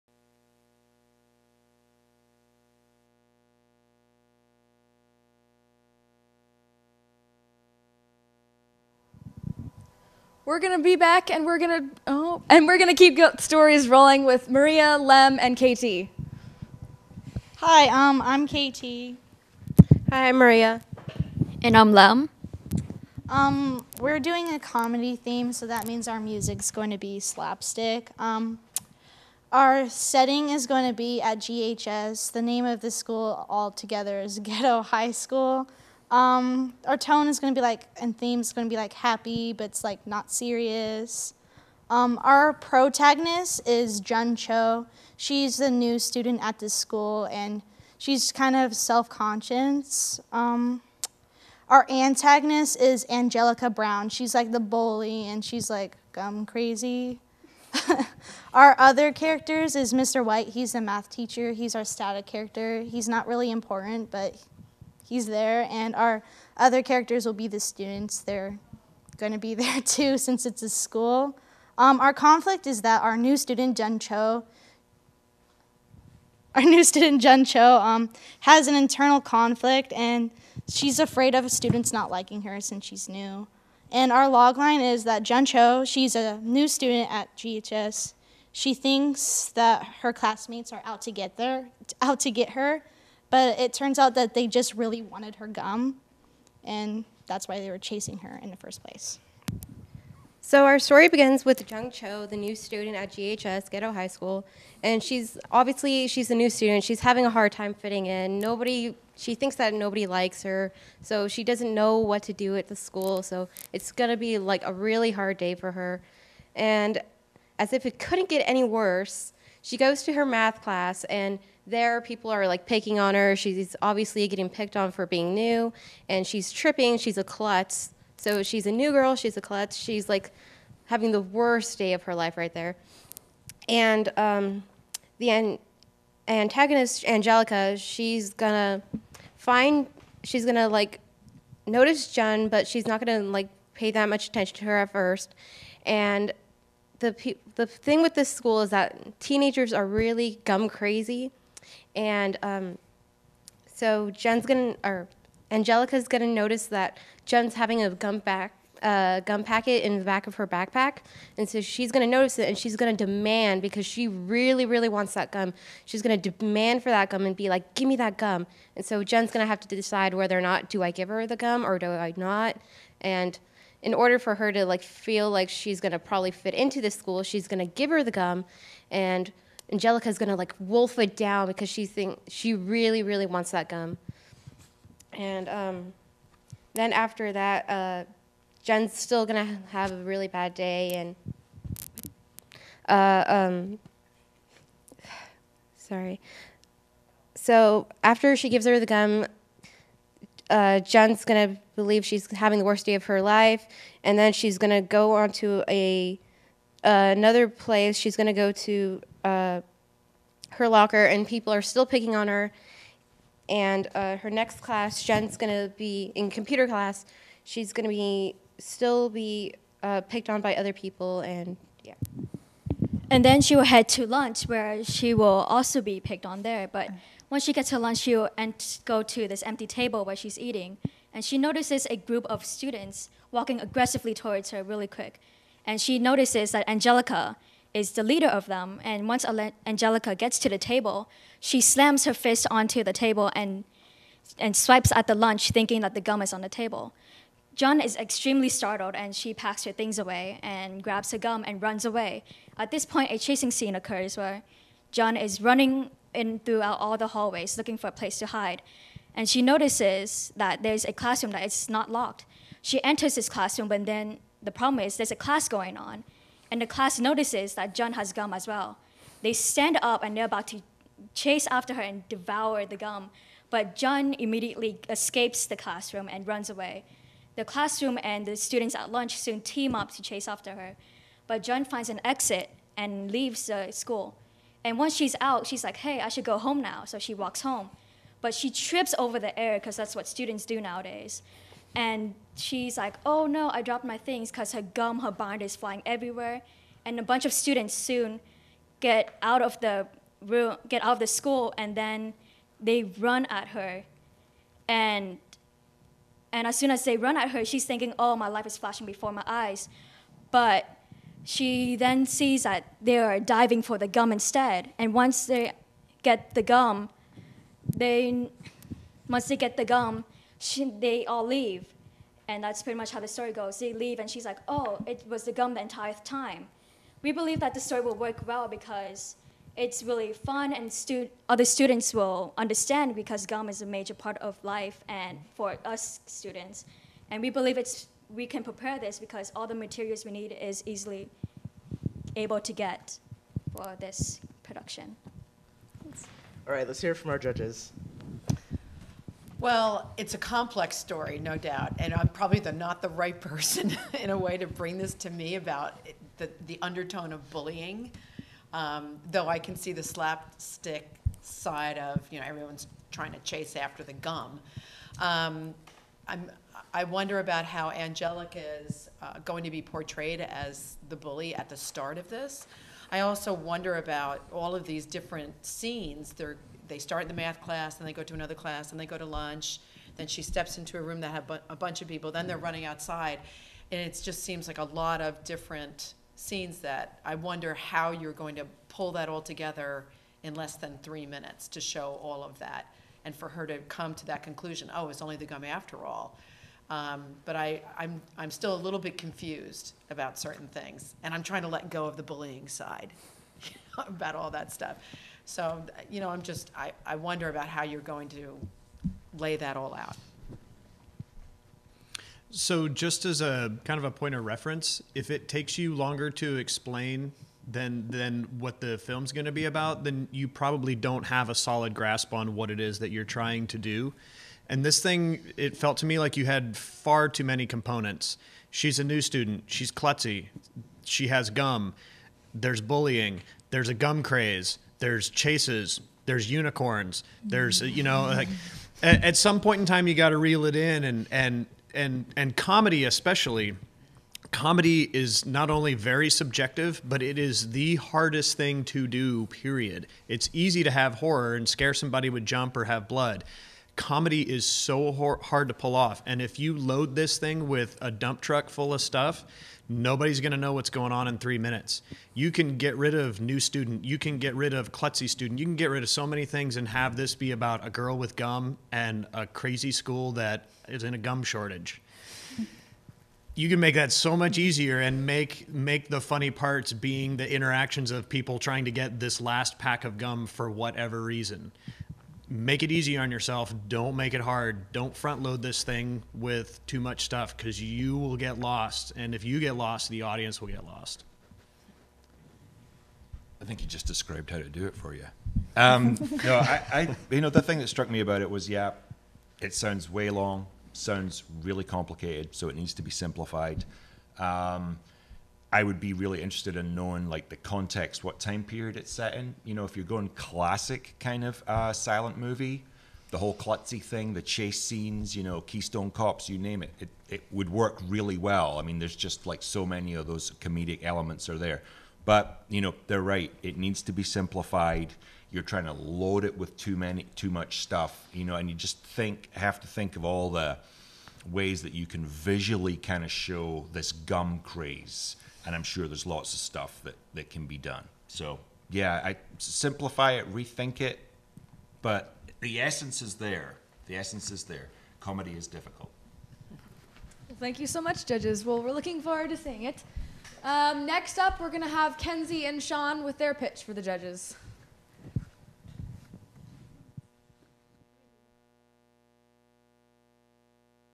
Pitch Perfect - KT, Maria and Lem
Students from the Center for Advanced Learning pitch their ideas for the CAL Five Minute Film Festival. Recorded at the MetroEast studios in Gresham.